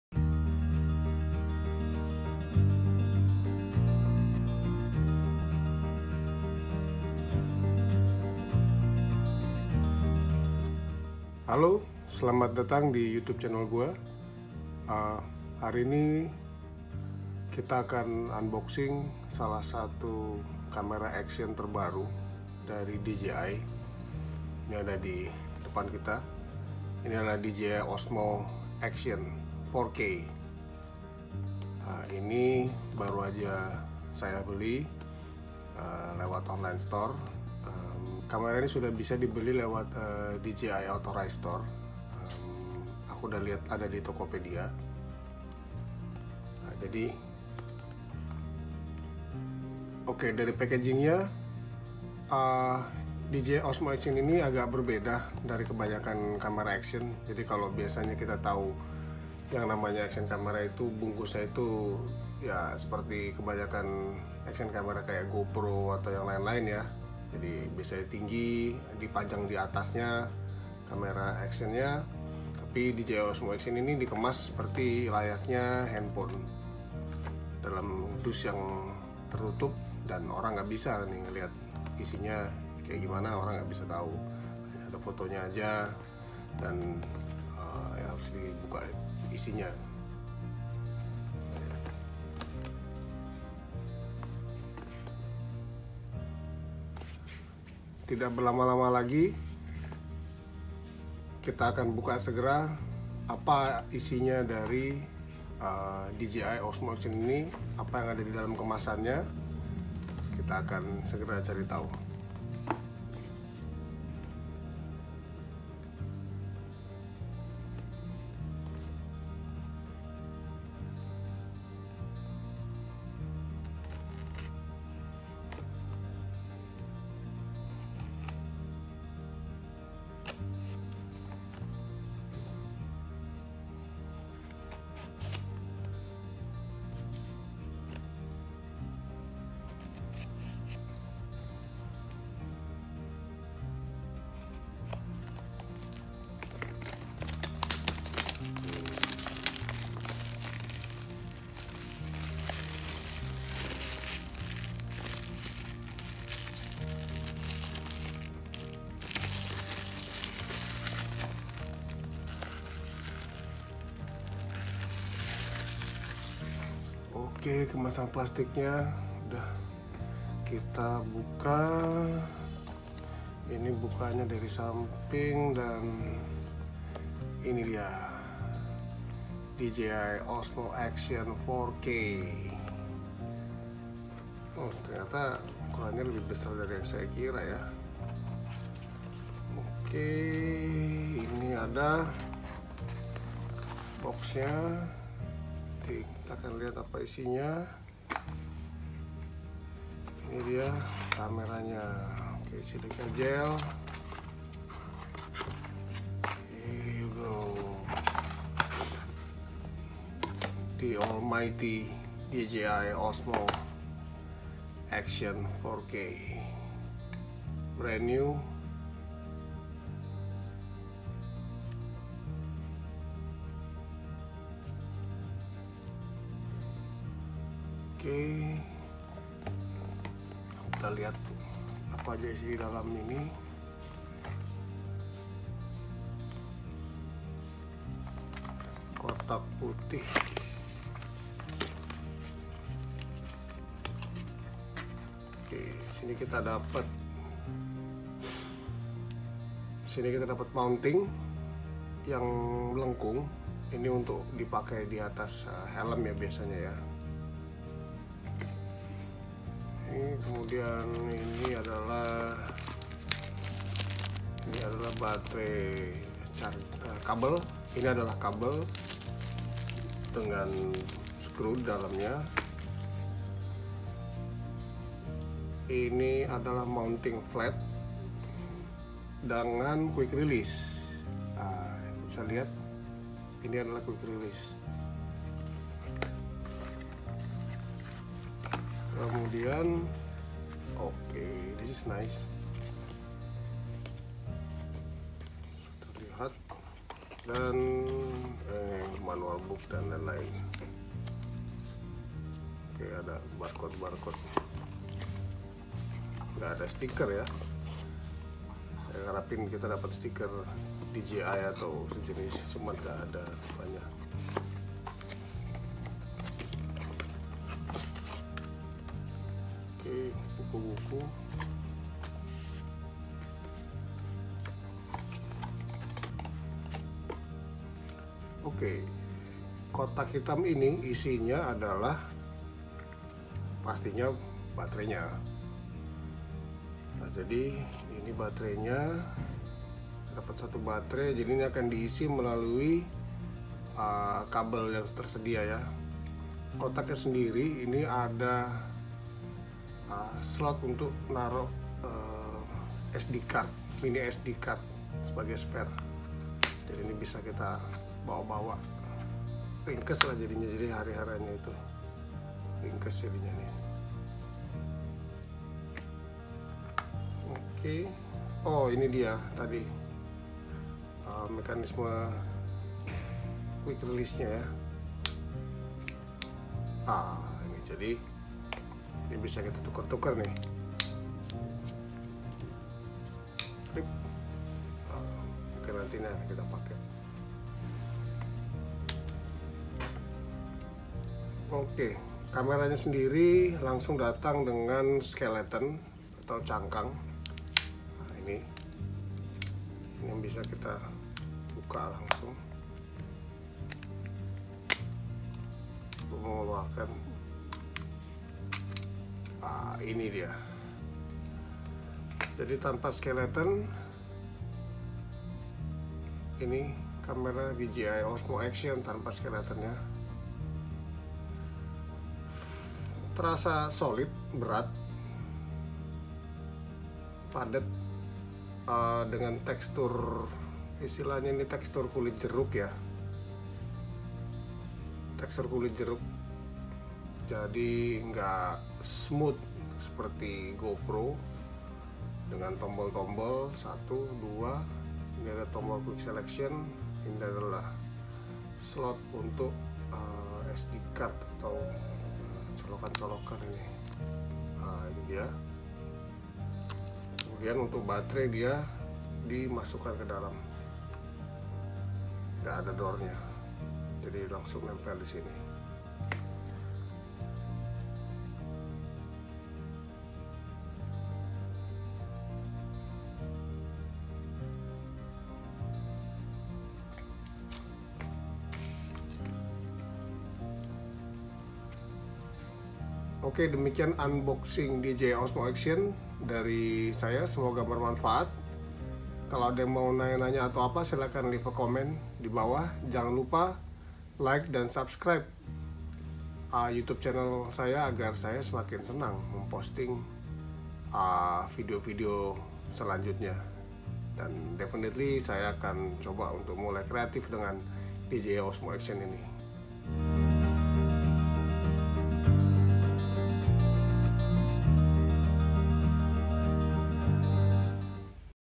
Unboxing DJI OSMO ACTION Indonesia !
DJI baru saja meluncurkan action camera pertamanya yaitu DJI Osmo Action. Bagaimana bentuknya, apa saja yang akan anda dapatkan di dalam box kemasannya ? Tonton sampai habis unboxing Osmo Action ini yaaaa!!